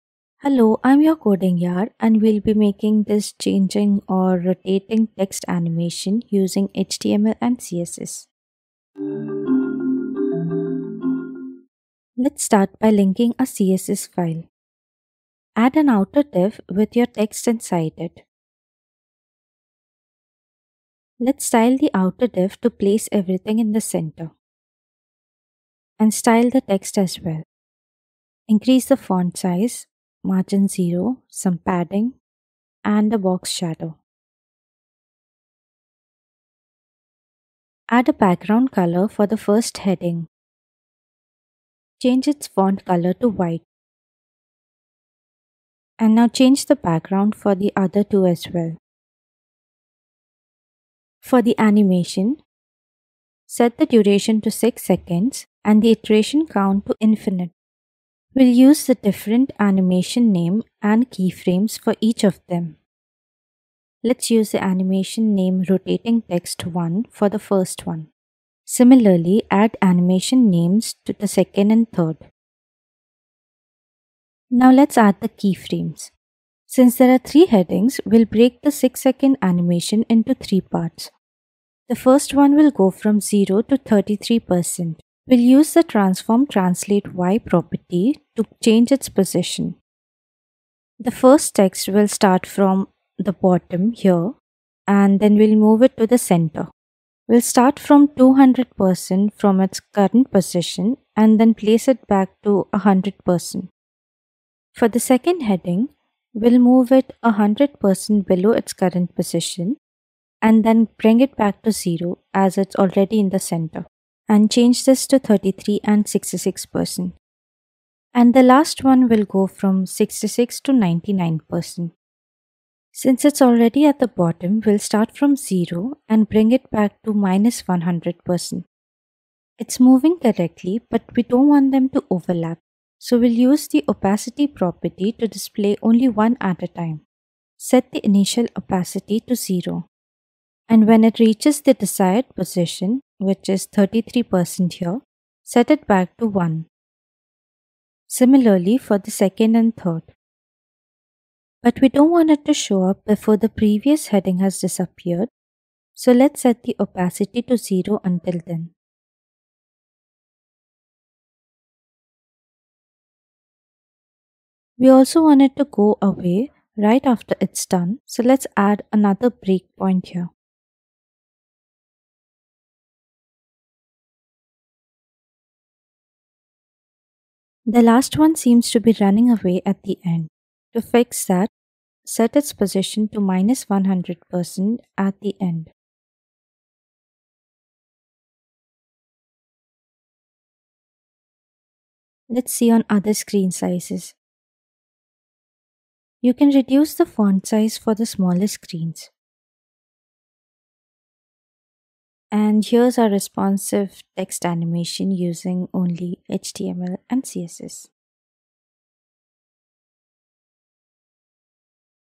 Responsive Text Animation using HTML and CSS (changing or rotating)
Here’s a simple way to create a responsive changing or rotating text animation using HTML and CSS only.

HTML | CSS

Timestamps:
0:00 - Final Output
0:09 - Channel Intro
0:12 - Add text
0:21 - Outer div and text stying
0:50 - Add CSS animation to the text
1:15 - Add keyframes
3:11 - Responsiveness